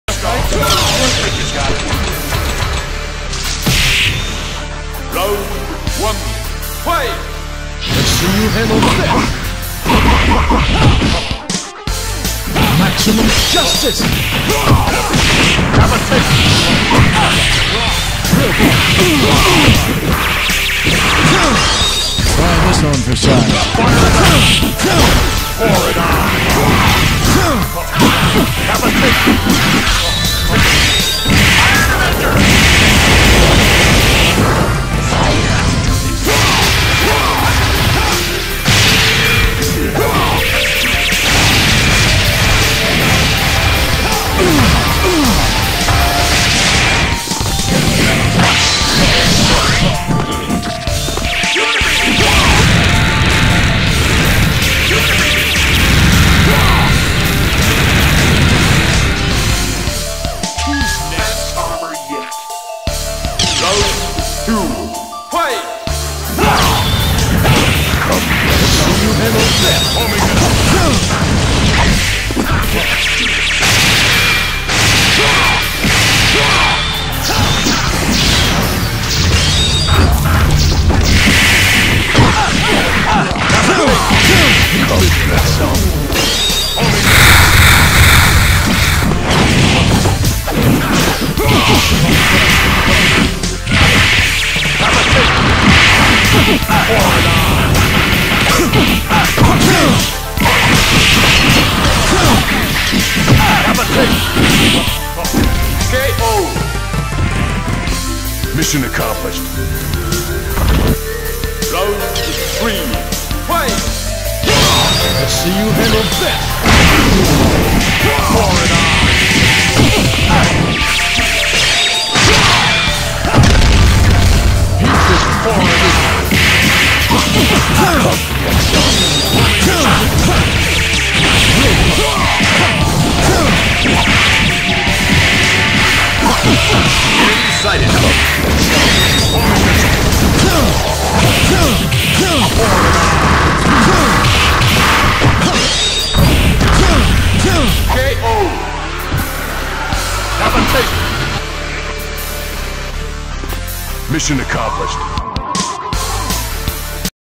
Justice League Vs. Avengers
Justice League Vs. Avengers : AI BATTLE : 3 vs 3

I do not own the BGM used in this Video.